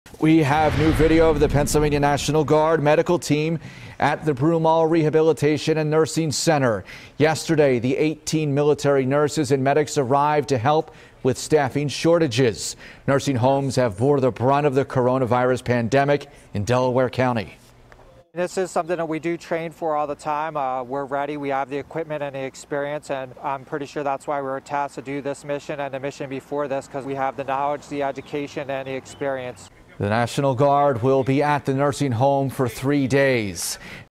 Pennsylvania National Guard Medical Team Arrives At Broomall Rehabilitation And Nursing Center
The 18 military nurses and medics will help out for three days.